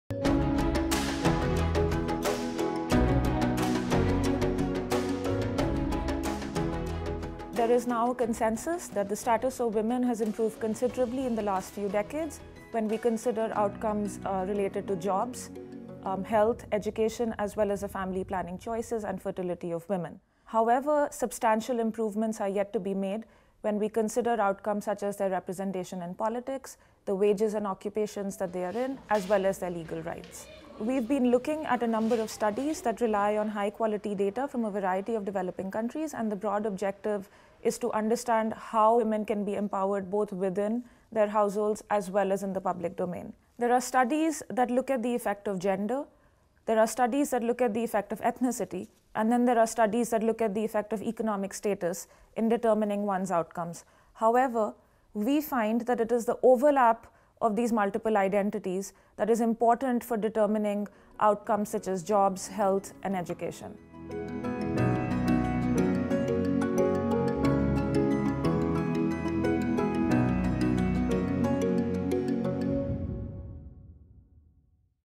GENDER: Lessons for Development
What does the last decade of research at United Nations University World Institute for Development Economics Research (UNU-WIDER) tell us about GENDER?

This is part of a series of videos on Lessons for Development to mark the last two Work Programmes at UNU-WIDER.